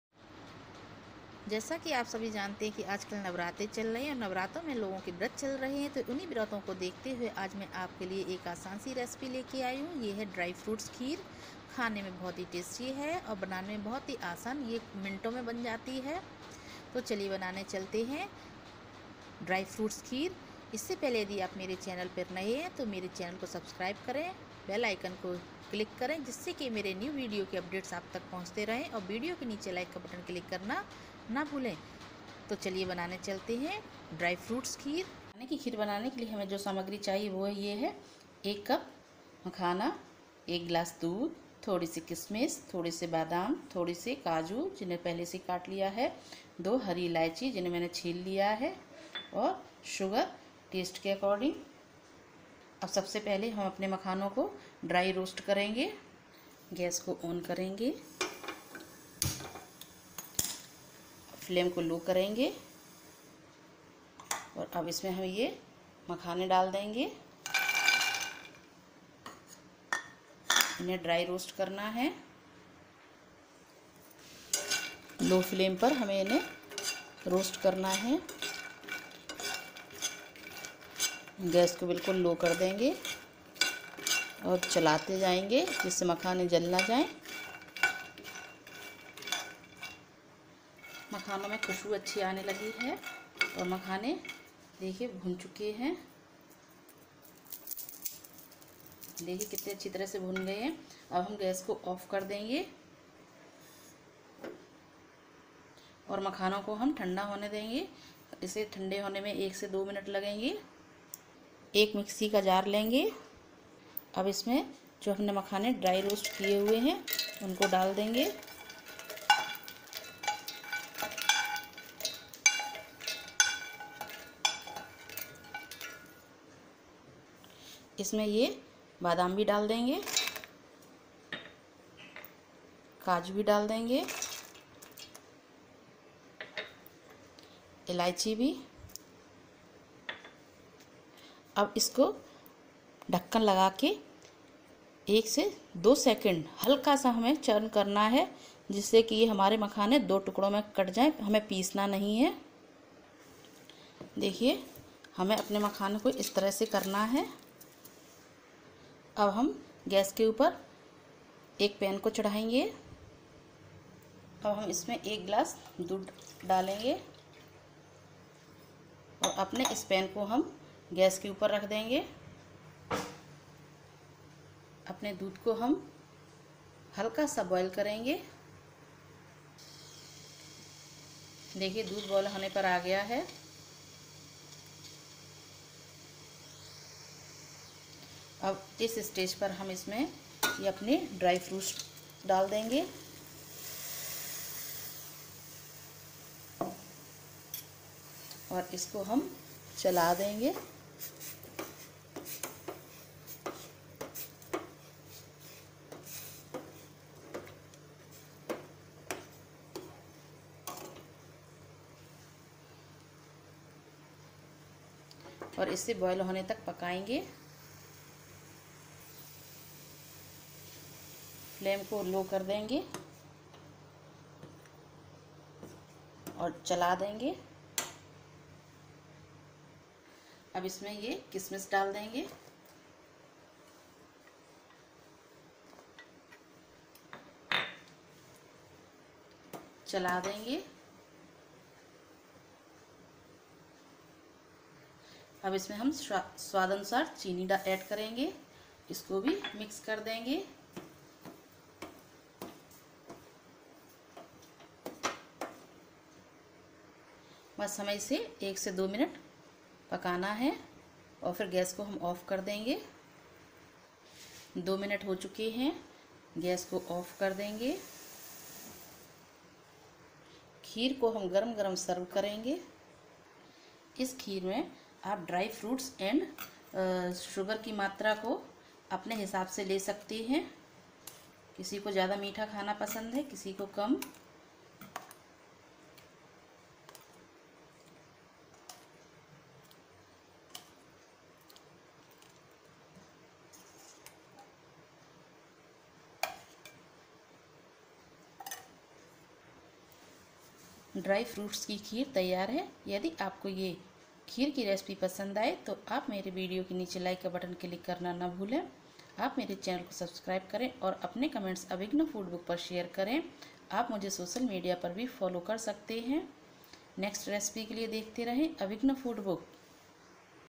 Dry Fruit Kheer / Super Delicious Mewa ki Kheer
Dry fruits kheer have various  nutrients and easy to make. you can make this recipe with in 5 minutes. Dry fruits kheer is the best recipes for vrat.

Ingredients :
1 cup Makhana
1 glass milk
25- Raisins
15 Almonds
15 cashew nuts
2 green cardmum

suggestion
you can adjust the quantity of dry fruits and sugar as per your taste.